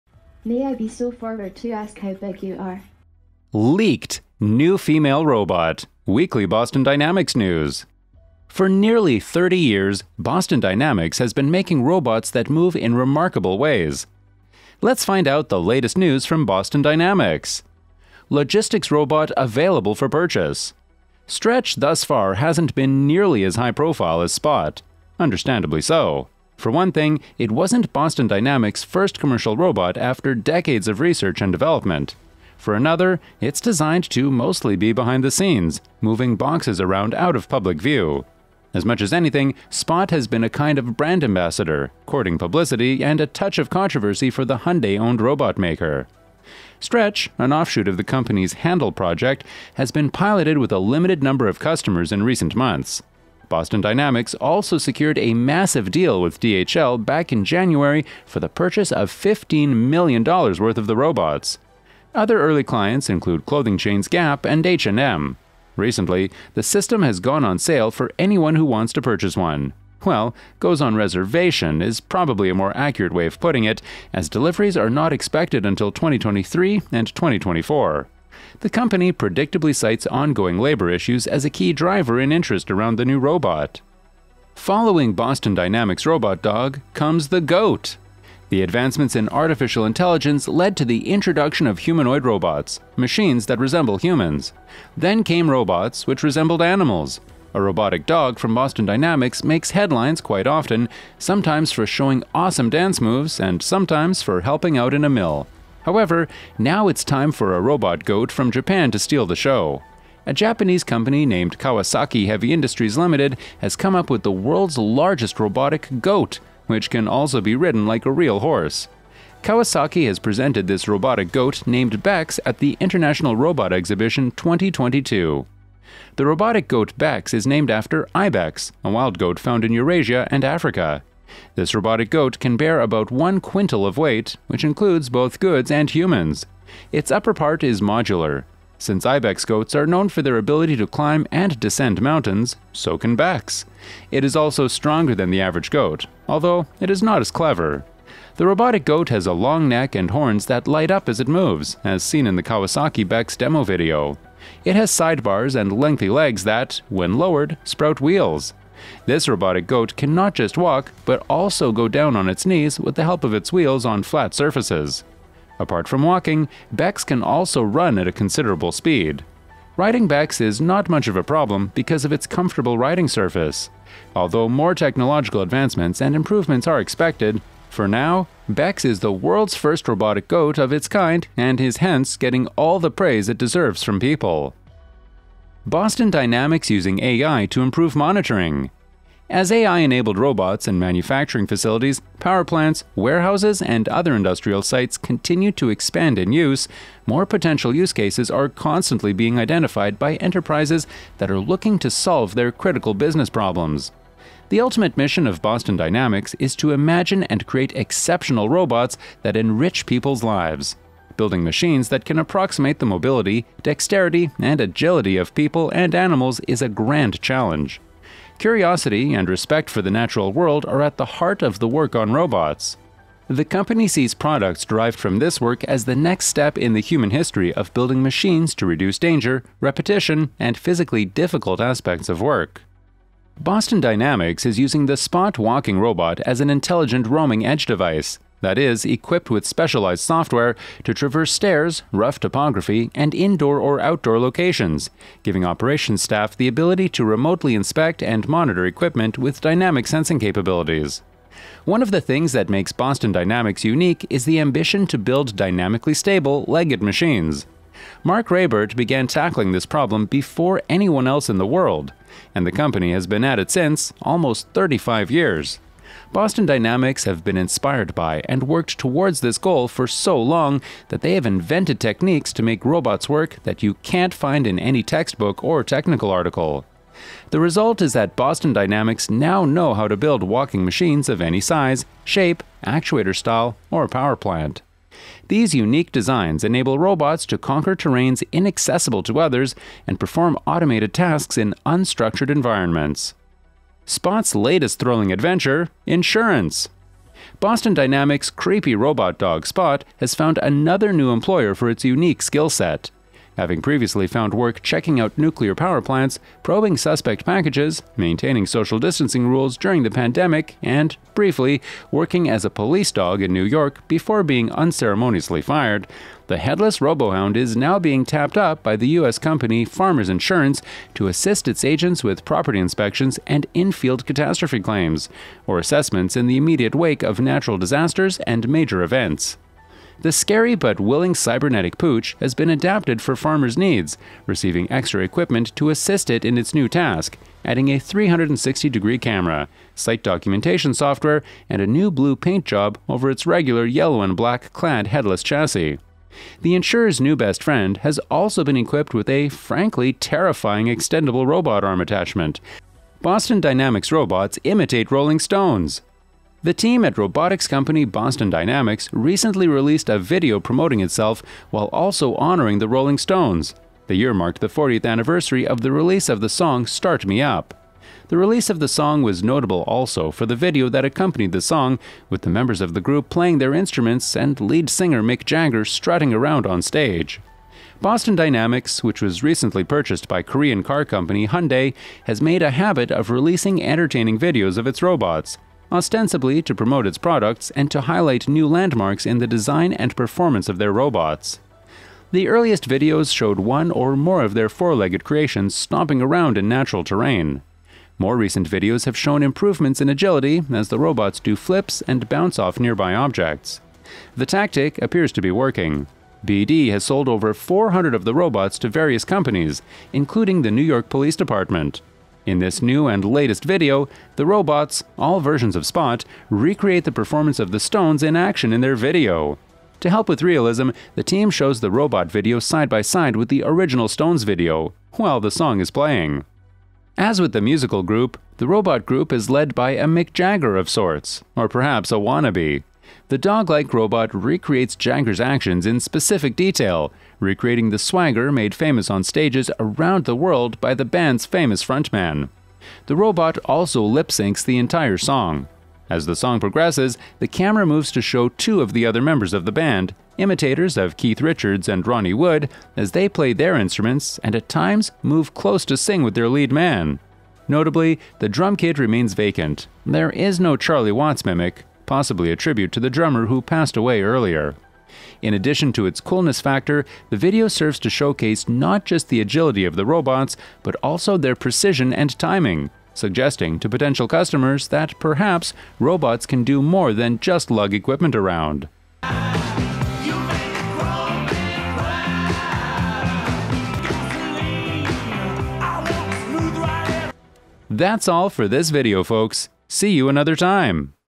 Boston Dynamics JUST REVEALED NEW Female Humanoid Robot
Every week we will update you about the latest news regarding Boston Dynamics. Logistics Robot Available for Purchase
Stretch thus far hasn’t been nearly as high-profile as Spot. Understandably so. For one thing, it wasn’t Boston Dynamics’ first commercial robot after decades of research and development. For another, it’s designed to mostly be behind the scenes, moving boxes around out of public view. As much as anything, Spot has been a kind of brand ambassador, courting publicity and a touch of controversy for the Hyundai-owned robot maker.
Stretch, an offshoot of the company’s Handle project, has been piloted with a limited number of customers in recent months. Boston Dynamics also secured a massive deal with DHL back in January for the purchase of $15 million worth of the robots. Other early clients include clothing chains Gap and H&M. Recently, the system has gone on sale for anyone who wants to purchase one. Well, goes on reservation is probably a more accurate way of putting it, as deliveries are not expected until 2023 and 2024. The company predictably cites ongoing labor issues as a key driver in interest around the new robot.

Following Boston Dynamics Robot Dog, Comes The GOAT!
The advancements in artificial intelligence led to the introduction of humanoid robots, machines that resemble humans. Then came robots, which resembled animals. A robotic dog from Boston Dynamics makes headlines quite often, sometimes for showing awesome dance moves and sometimes for helping out in a mill. However, now it’s time for a robot goat from Japan to steal the show. A Japanese company named Kawasaki Heavy Industries Limited has come up with the world’s largest robotic goat, which can also be ridden like a real horse. Kawasaki has presented this robotic goat named Bex at the International Robot Exhibition 2022.
The robotic goat Bex is named after Ibex, a wild goat found in Eurasia and Africa. This robotic goat can bear about 1 quintal of weight, which includes both goods and humans. Its upper part is modular. Since Ibex goats are known for their ability to climb and descend mountains, so can Bex. It is also stronger than the average goat; although it is not as clever. The robotic goat has a long neck and horns that light up as it moves, as seen in the Kawasaki Bex demo video. It has sidebars and lengthy legs that, when lowered, sprout wheels.

Boston Dynamics Using AI to Improve Monitoring
As AI-enabled robots in manufacturing facilities, power plants, warehouses, and other industrial sites continue to expand in use, more potential use cases are constantly being identified by enterprises that are looking to solve their critical business problems. The ultimate mission of Boston Dynamics is to imagine and create exceptional robots that enrich people’s lives. Building machines that can approximate the mobility, dexterity, and agility of people and animals is a grand challenge.